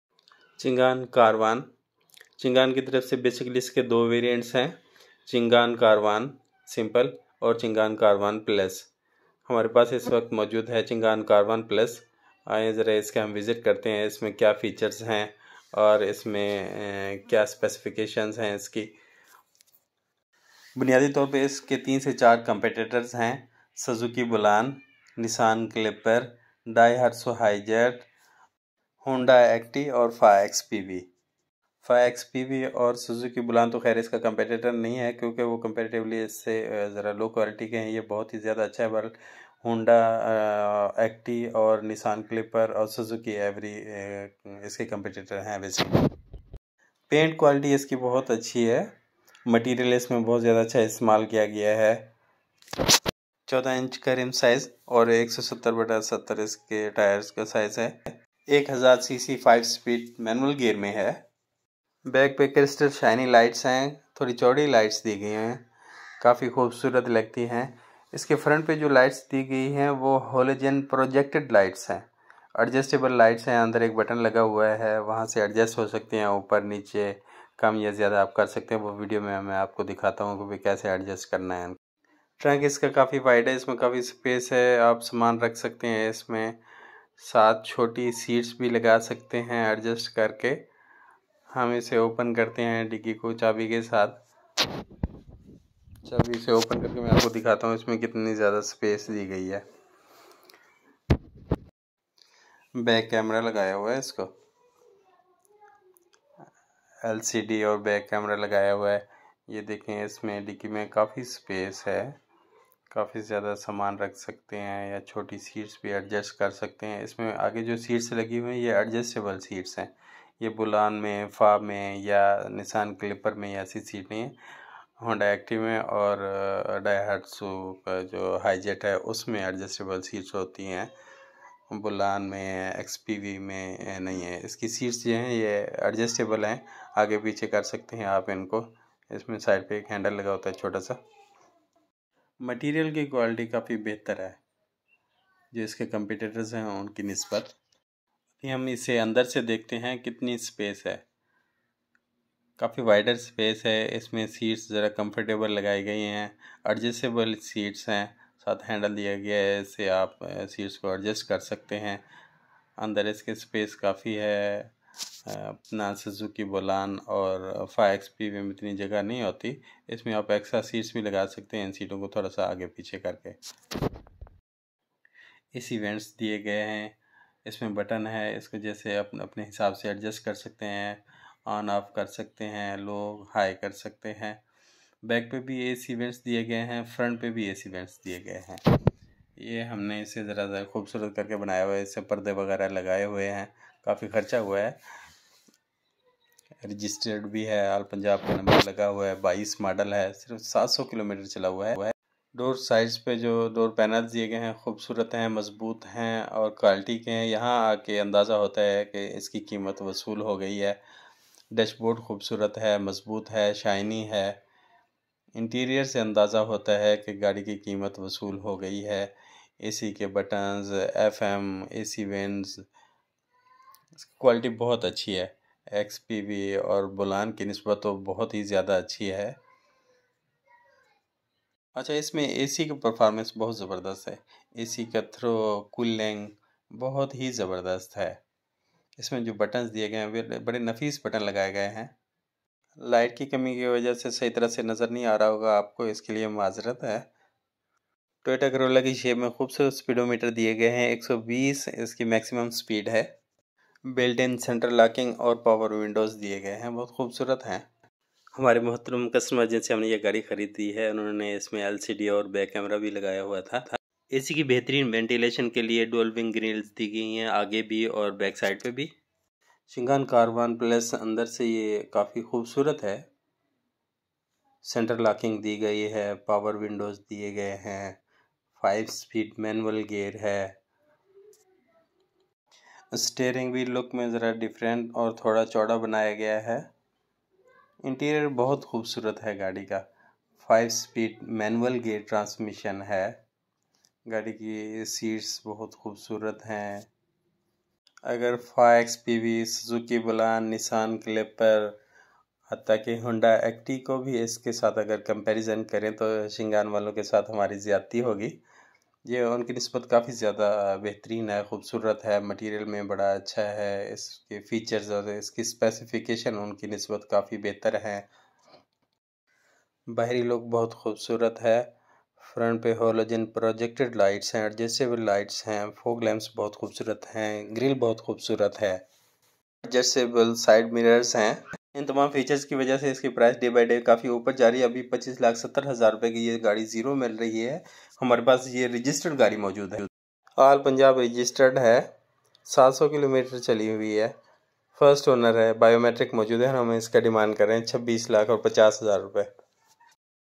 Changan Karvaan plus in Pakistan | karvaan price |Comparison among karvan, hijet, acty and cliper
Changan karvaan plus price in pakistan
changan karvaan plus specifications
Changan karvaan plus features